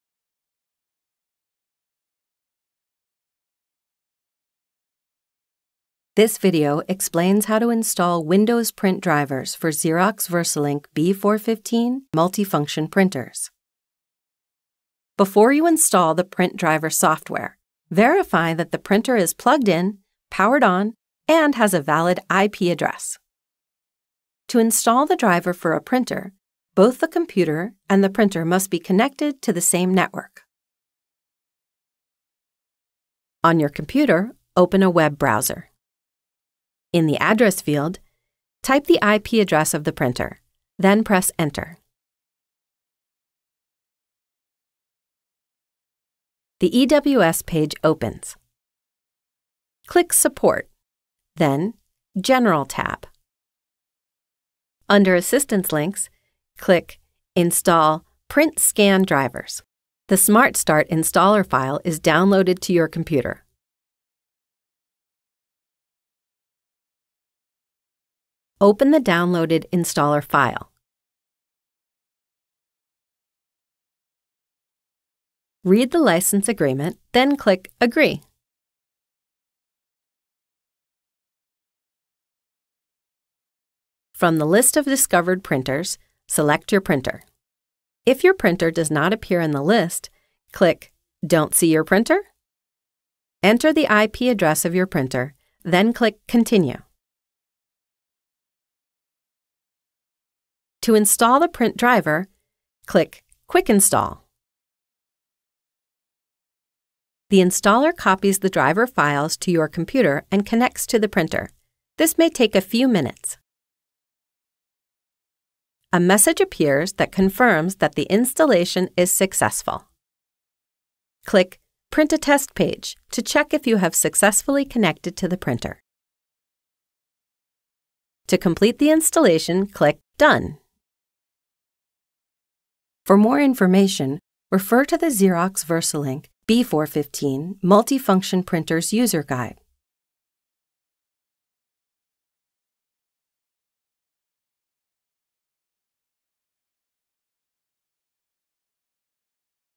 Xerox® VersaLink® B415 Multifunction Printer Install Print Drivers for a Windows Network Printer
This video demonstrates how to download a Print Driver from Xerox.com and install the driver on a Windows computer.

BR39187